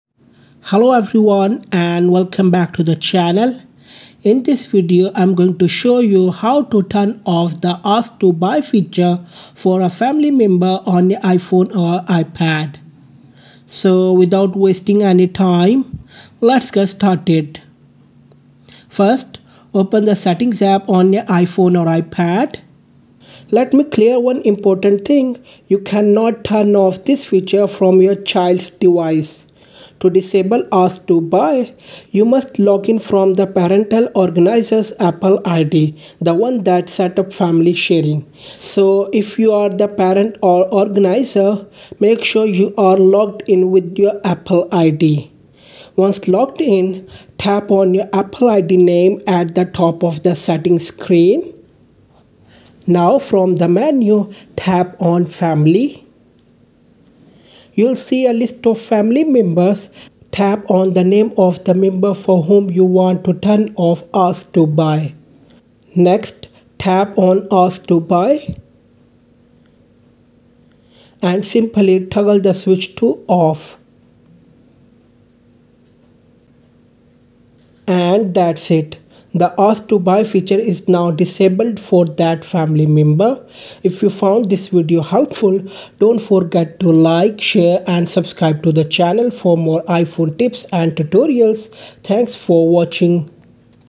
How to Turn Off Ask to Buy a Family Member on iPhone | Child Account Cannot Buy Apps on App Store
Learn how to easily disable the “Ask to Buy” feature for a family member on your iPhone or iPad using Family Sharing. This step-by-step tutorial will guide you through the process.

Topics Covered:
• Open Settings
• Go to Apple ID
• Family Sharing
• Select Family Member
• Turn Off Ask to Buy